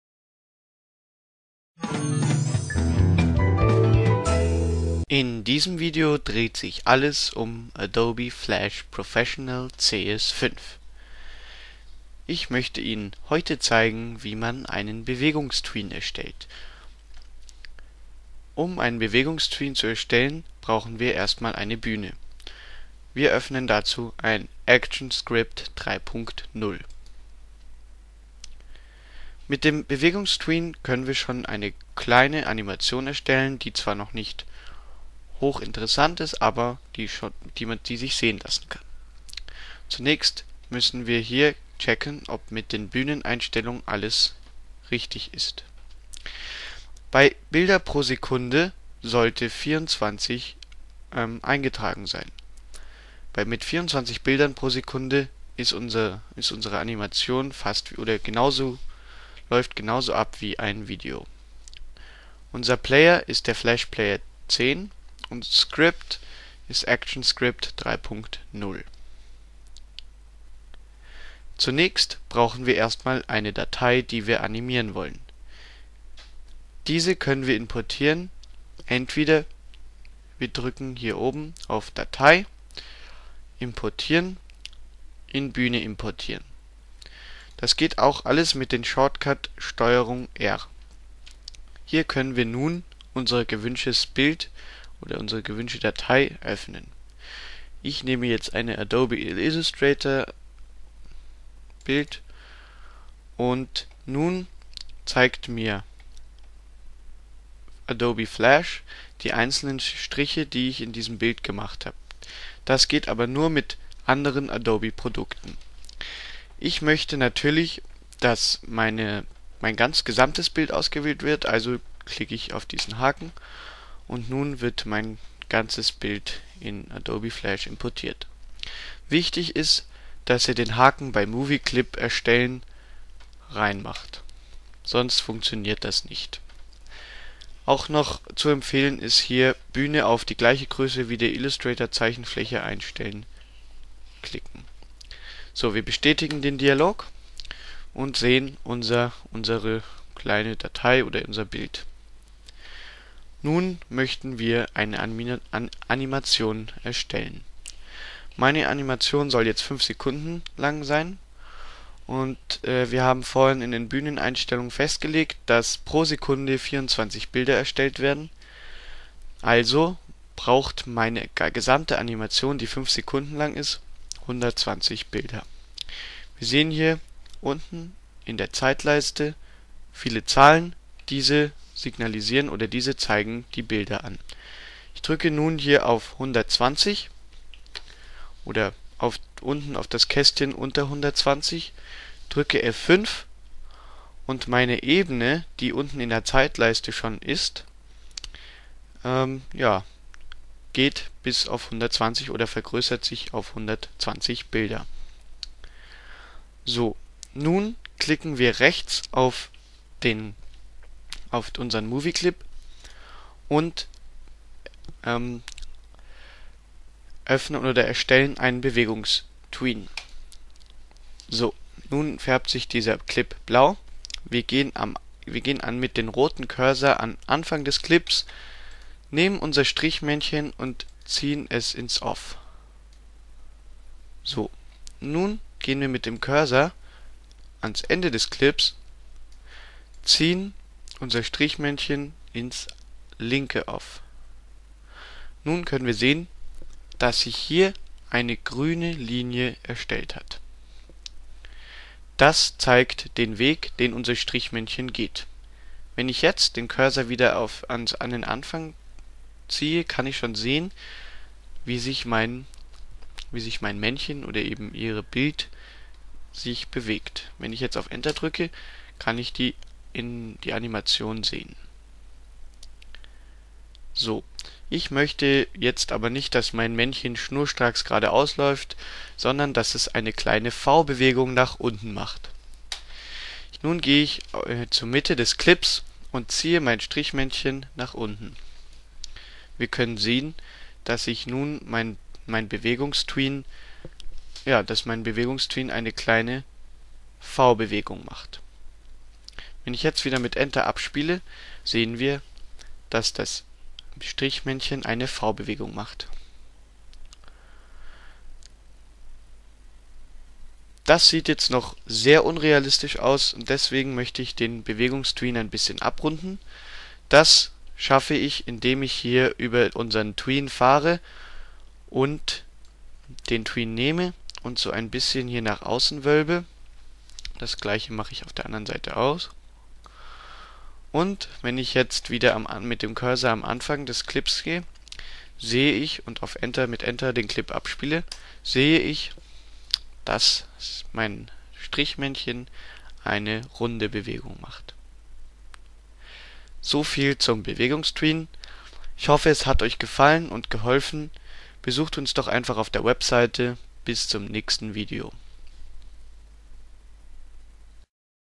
Einfache Animation in Adobe® Flash® CS4 oder CS5 ohne Kenntnisse
Dies ist ein Video, welches euch zeigt wie man mit Hilfe von Adobe® Flash® Professional einen simplen Bewgungstween erstellt.

Adobe product screen shot(s) reprinted with permission from Adobe Systems Incorporated.
Adobe, Flash are either registered trademarks or trademarks of Adobe Systems Incorporated in the United States and/or other countries.